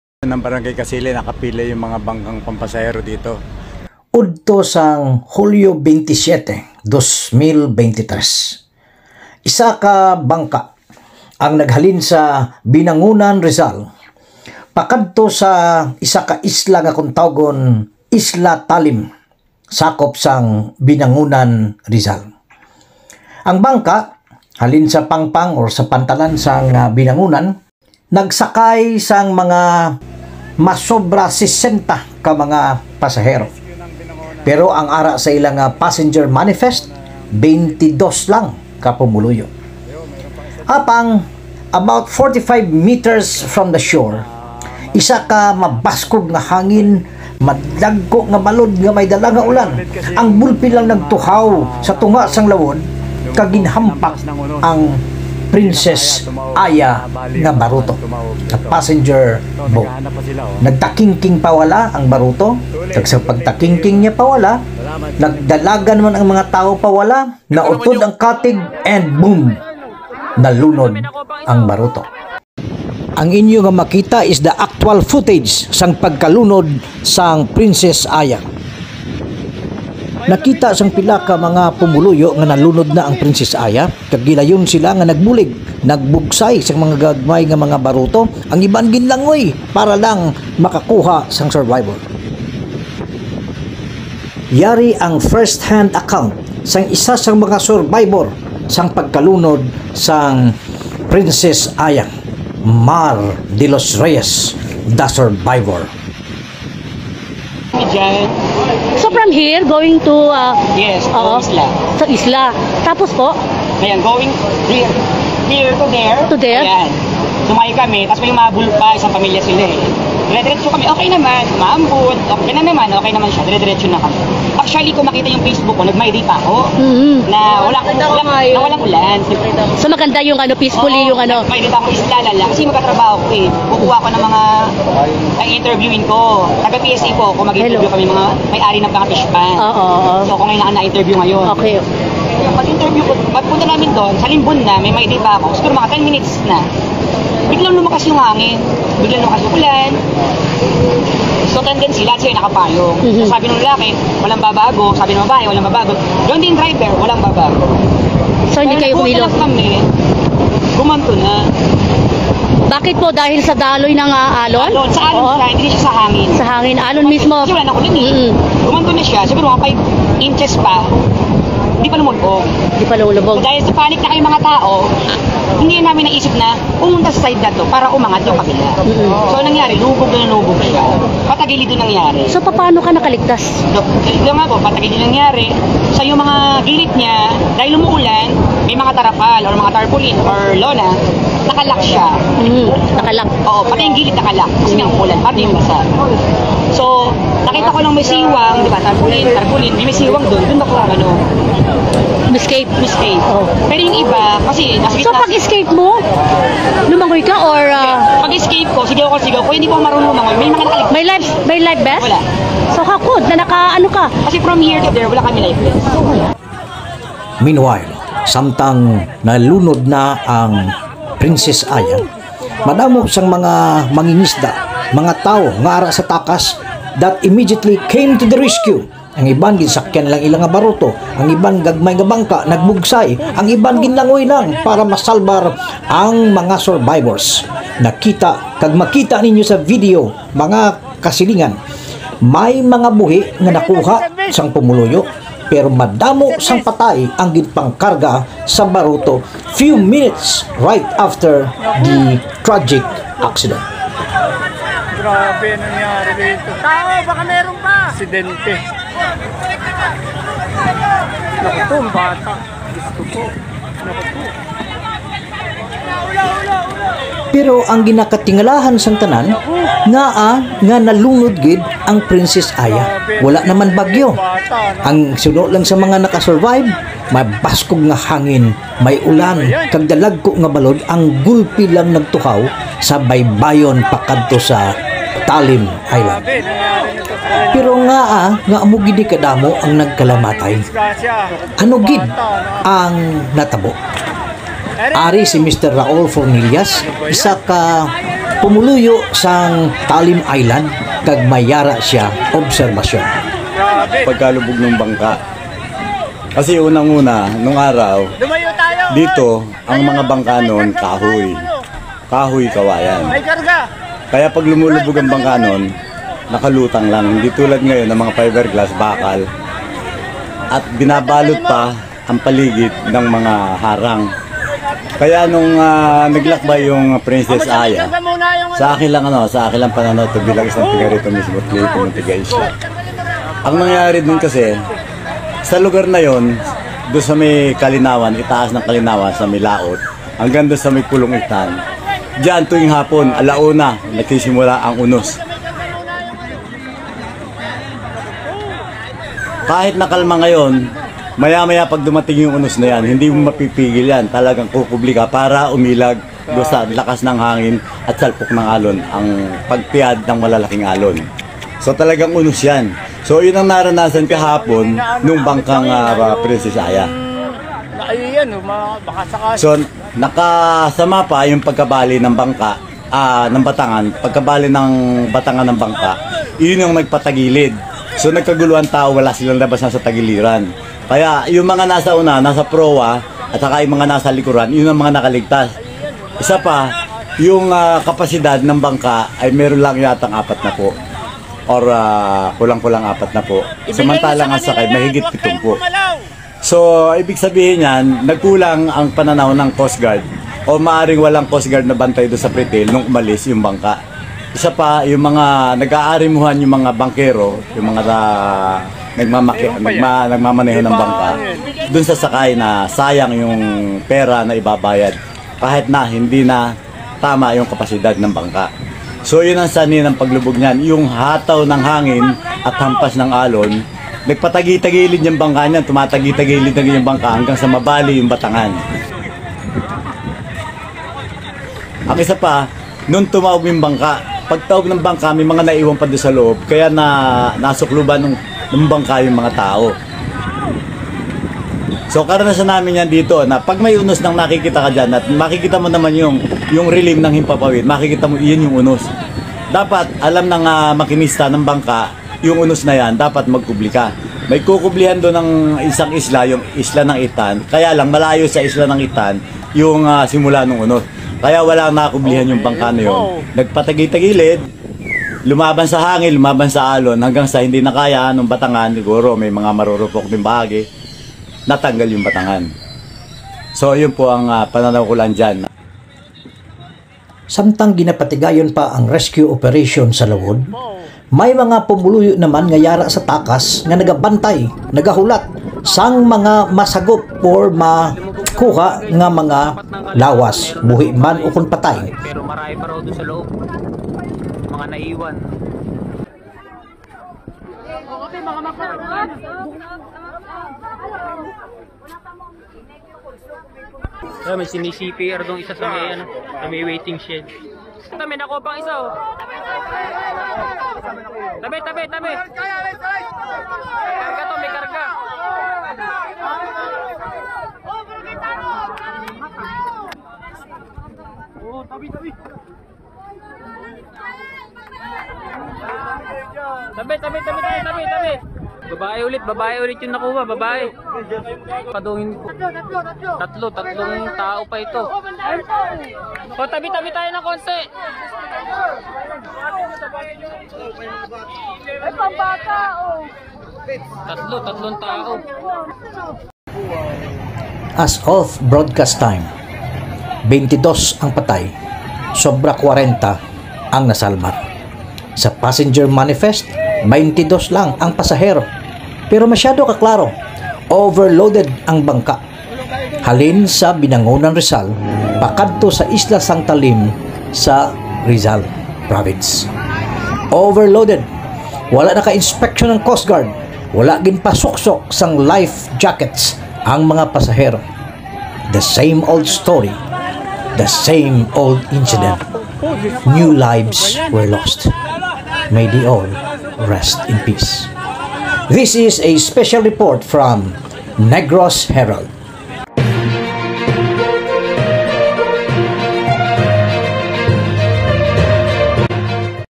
Binangonan RIZAL Sea Tragedy: The way to Talim Island Mystery
The sinking of Princess Aya.
The Talim Island mystery - BINANGONAN, RIZAL Tragedy.